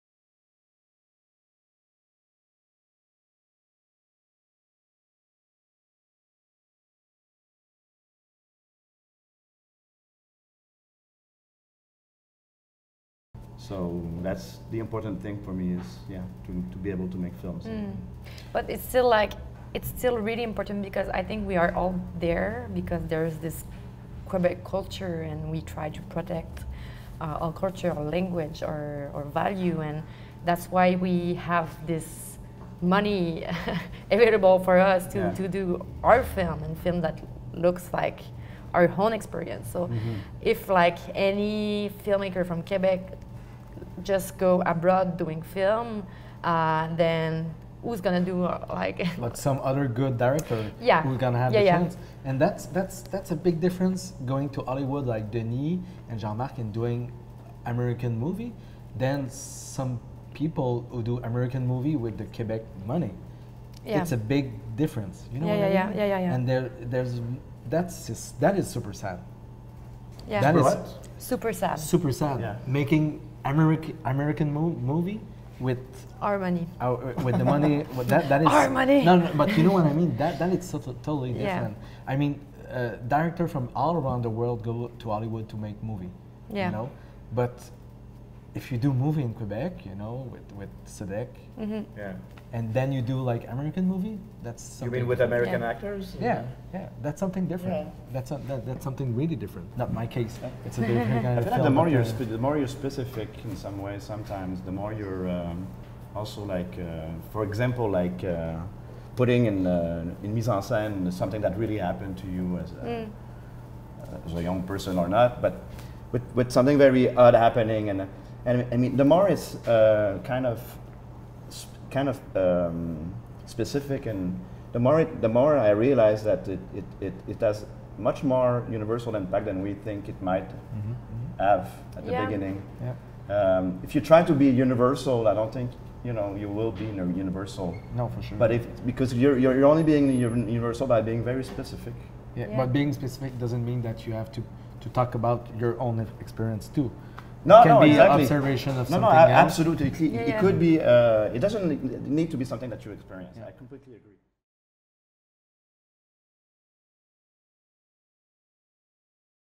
Quebec on Screen Ep. 4 - Making English Films Pt. 2
Geneviève Dulude-De Celles, Philippe Lesage and Maxime Giroux expand on French filmmakers creating English films.